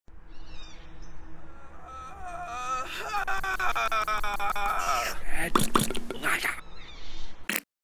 Dead In the Water
This is a parody of A video game cover for Dead In The Water, for the first playstation